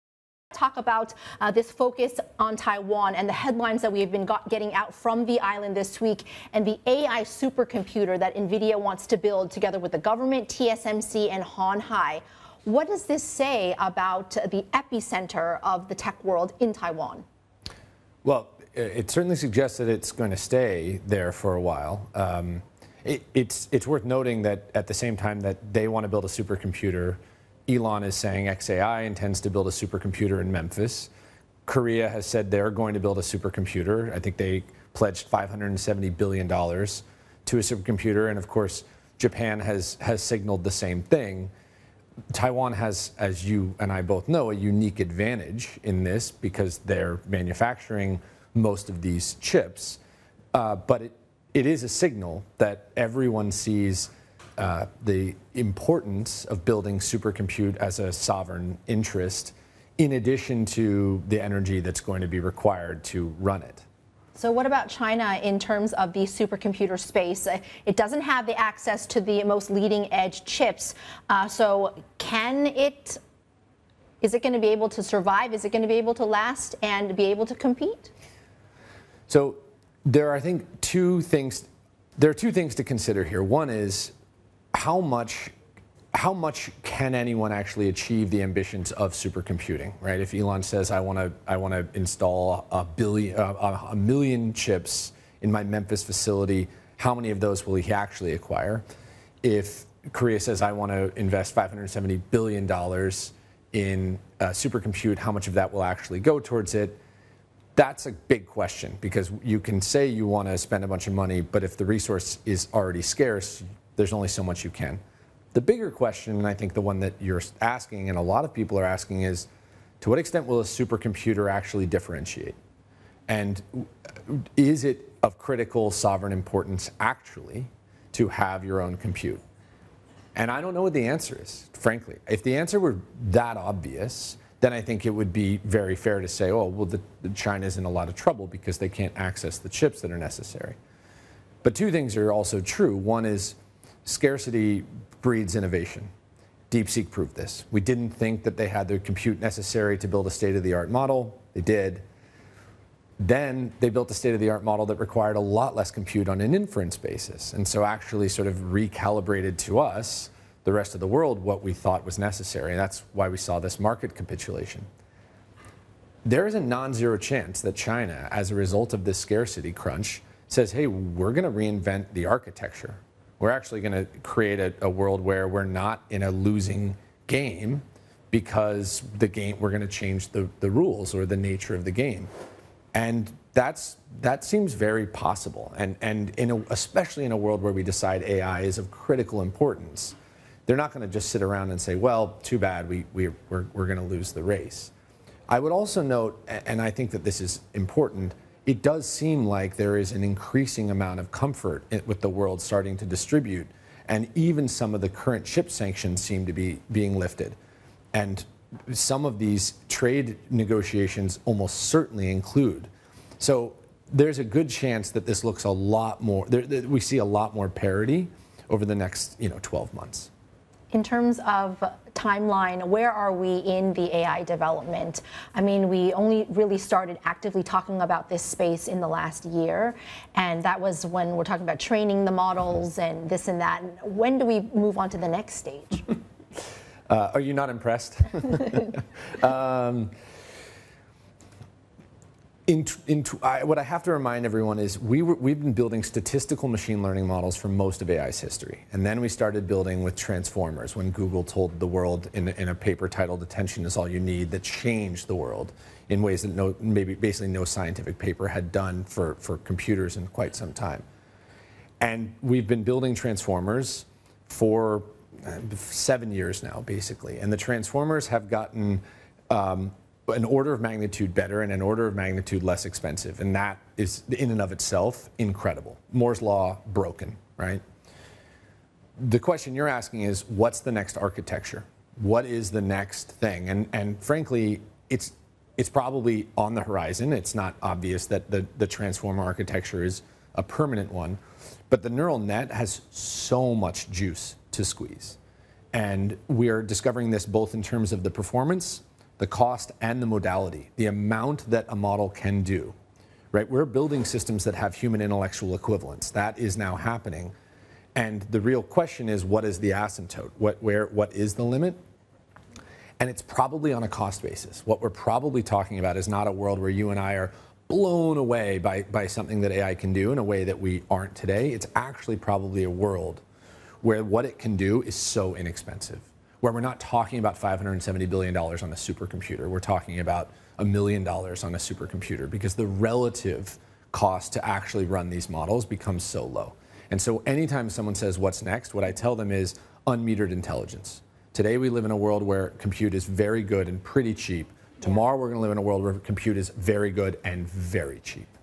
China could 'reinvent' AI architecture: Zack Kass
Zack Kass, AI keynote speaker and formerly head of Go To Market for OpenAI, talks about the global AI race, and how China's Deepseek moment is emblematic of what companies & countries are prioritizing in the global AI race. He says the biggest AI innovations to come will be on the cost side, as companies look to make it cheaper and easier to access.